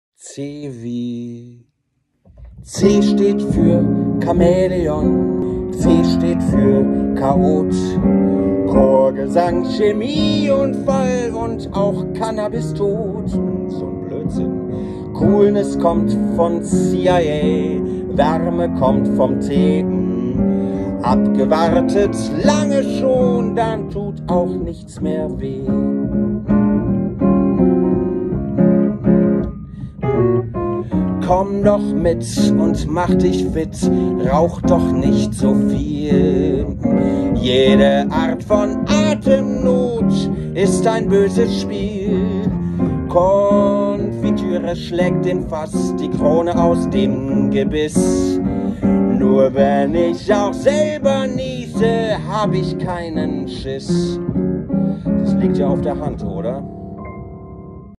Was passiert, wenn man die bösen C-Worte vermeiden möchte..
C wie...
C steht für Chamäleon, C steht für Chaot,
Chorgesang, Chemieunfall,
und auch Cannabis-Tod.

Ein Einfall im Oktober 2020